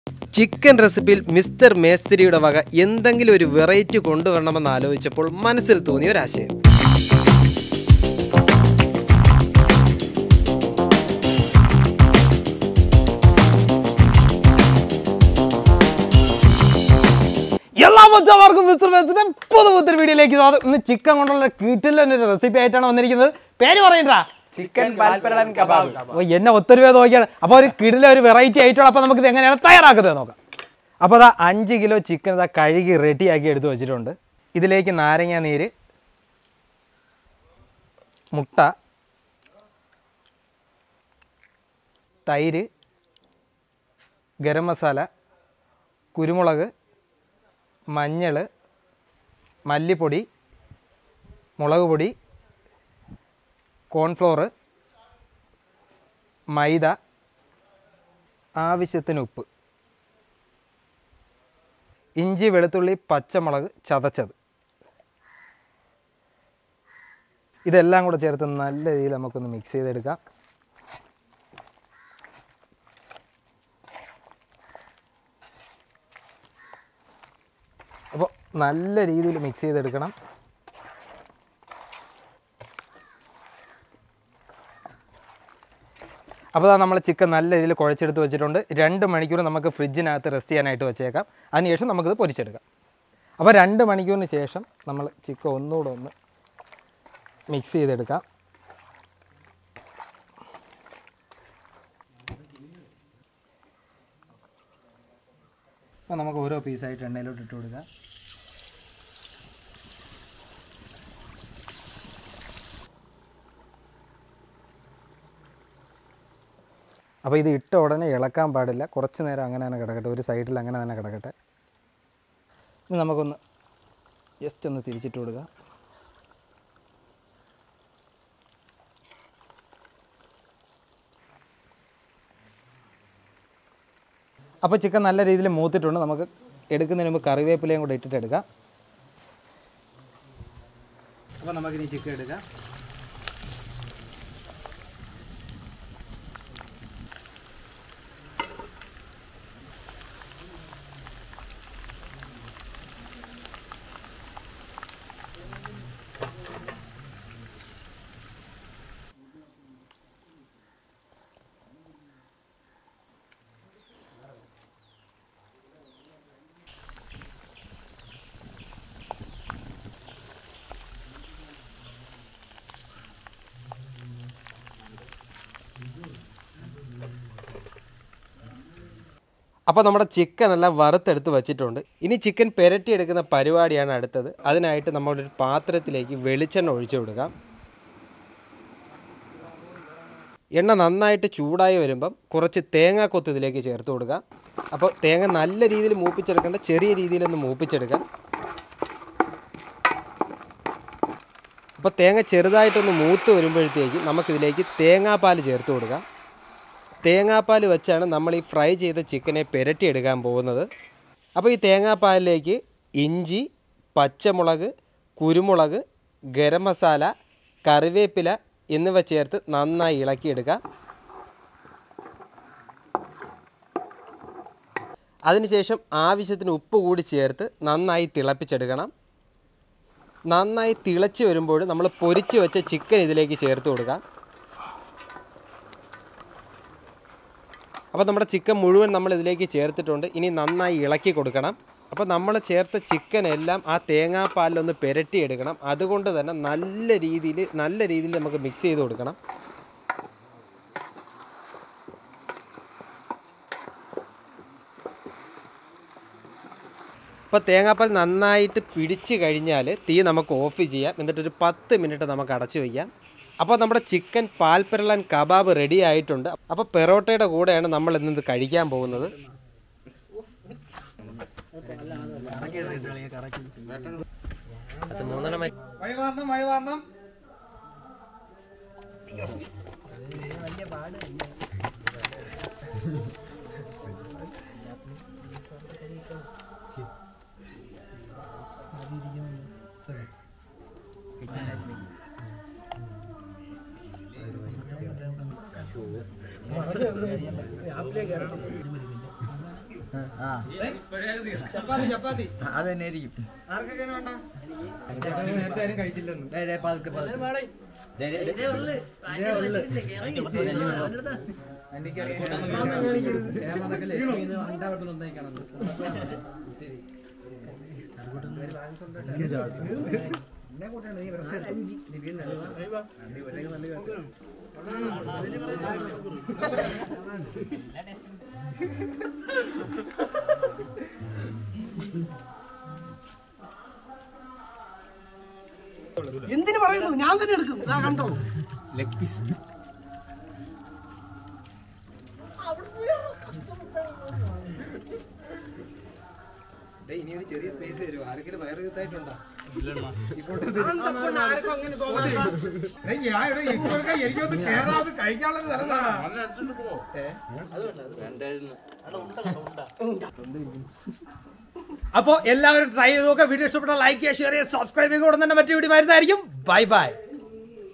ചിക്കൻ പാൽ പെരളൻ കബാബ് | Chicken Paal Peralan Kebab | A Special Chicken Recipe ❤😋
ചിക്കനിൽ പുതിയ ഒരു റെസീപ്പി കണ്ടുപിടിക്കണം എന്ന് ആലോചിച്ചപ്പോൾ മനസ്സിൽ വന്ന ഒരു ആശയമാണ് ചിക്കൻ പാൽ പെരളൻ കബാബ്. സത്യം പറഞ്ഞാൽ ഒരു അടിപൊളി സാധനം, എല്ലാവരും ട്രൈ ചെയ്ത് നോക്കുക